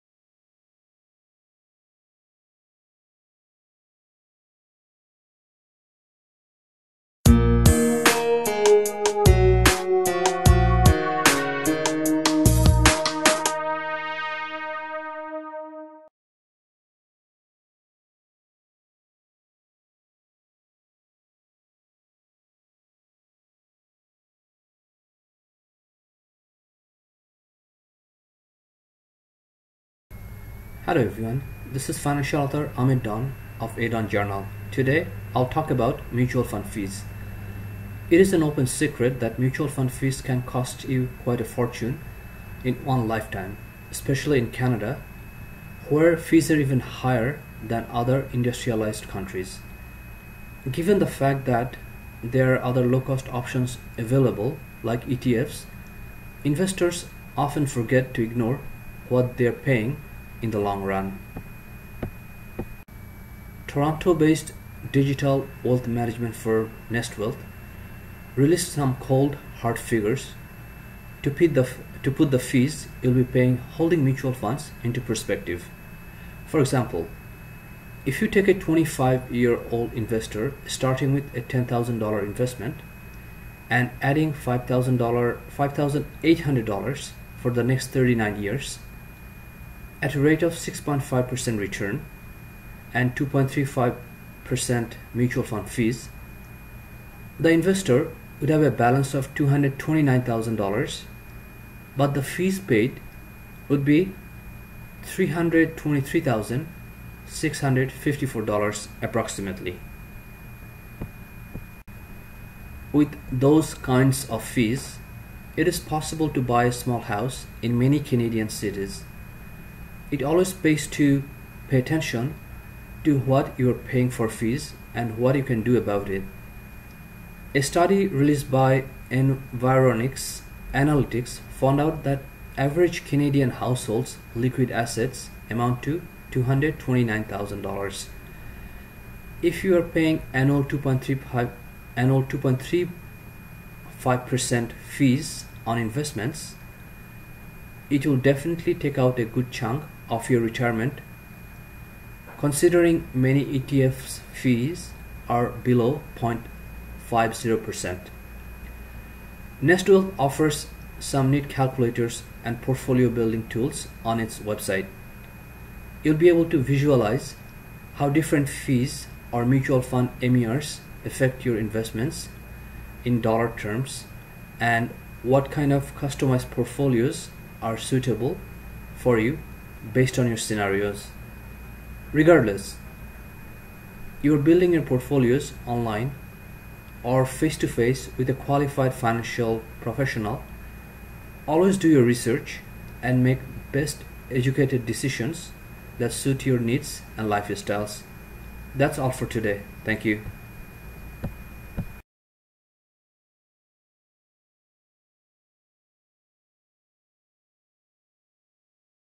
How to Avoid Paying Mutual Fund Fees | How to Invest in Mutual Funds | Financial Author Ahmed Dawn
How to Avoid Mutual Fund Fees. Canadian Financial author Ahmed Dawn's insights.